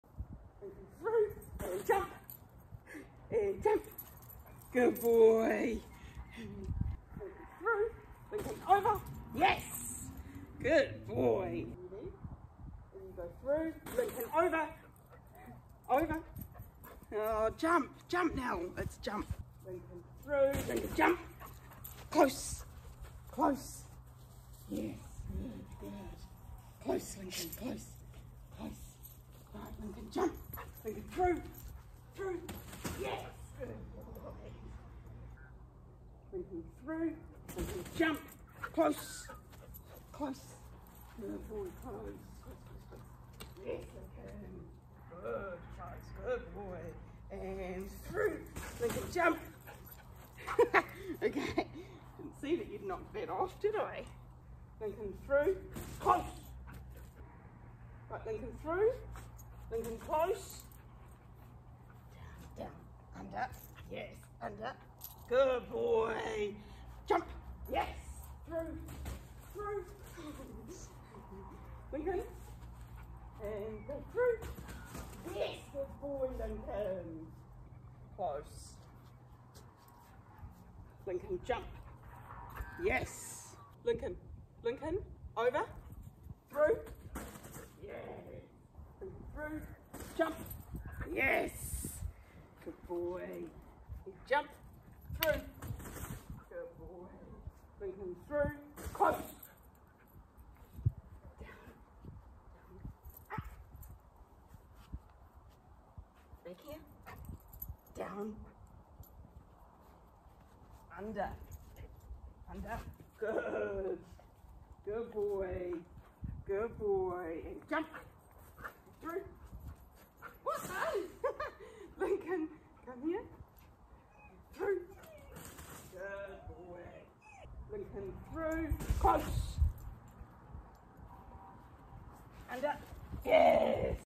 Lincoln doing agility
This is all too easy for Lincoln so he loses interest and doesn't pay attention. More obstacles required (and a better handler!).